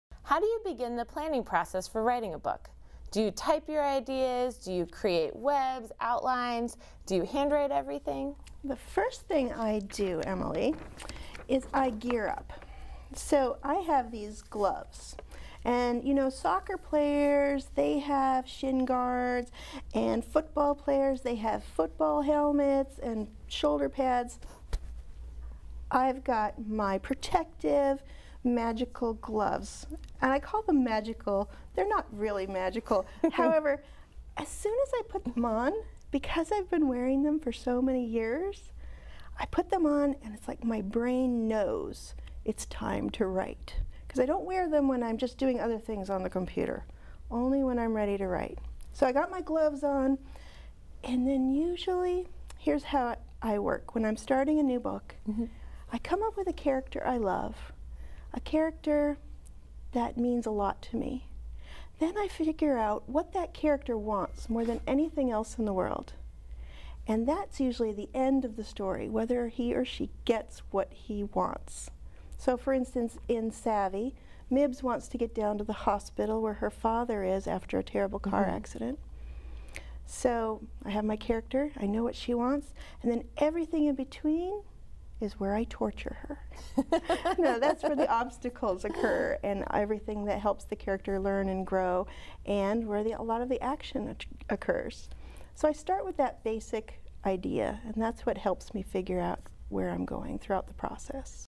Ingrid Law: "Gear Up" Gloves
Ingrid Law tells about her "gear up" gloves and her writing process.